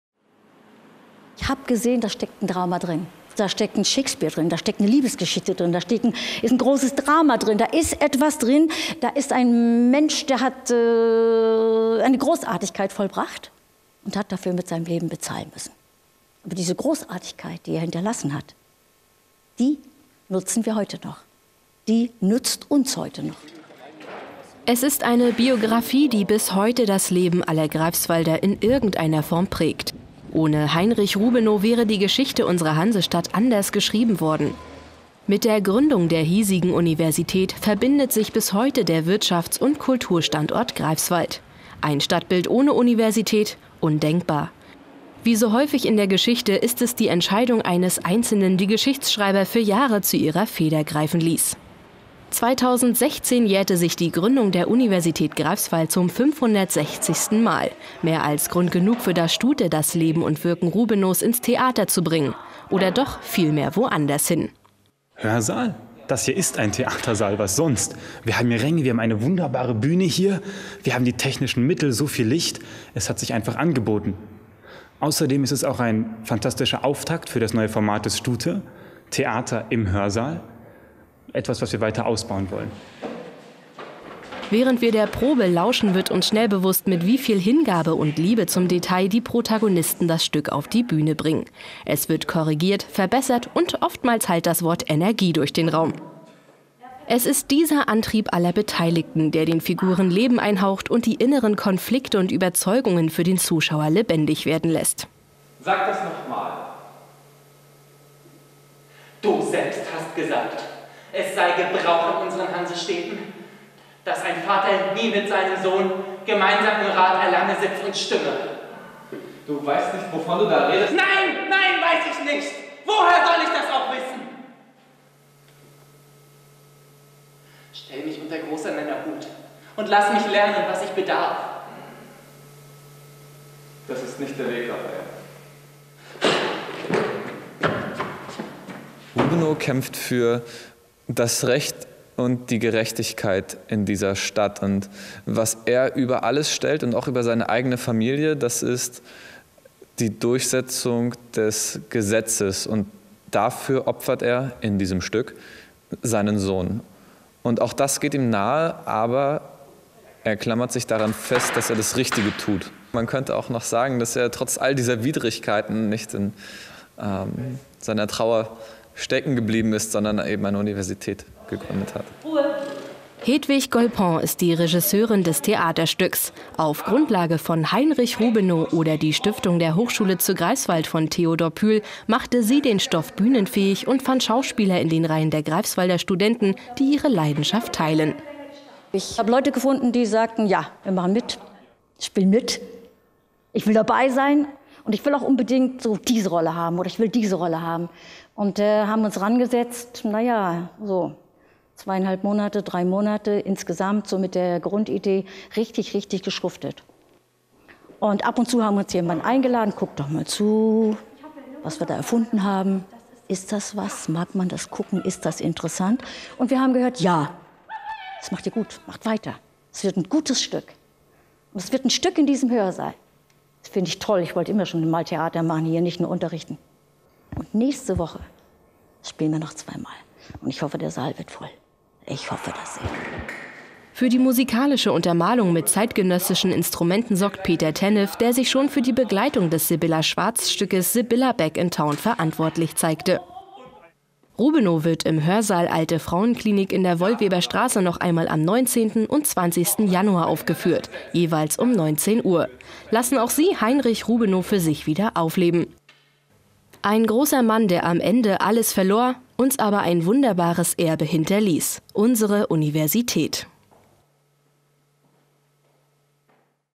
Rubenow der Unigründer
Möchte man die Geschichte Greifswalds erzählen, gibt es einige Namen, die unbedingt auftauchen müssen. Einer der Bedeutsamsten ist zweifelsohne Heinrich Rubenow. Das StuThe, das Studententheater, hat sein Leben und Wirken nun anlässlich des 560. Unigeburtstages auf die Bühne gebracht – oder vielmehr in den Hörsaal.
Redaktion: Robert Kindt
Kamera/ Schnitt: Nico Schröder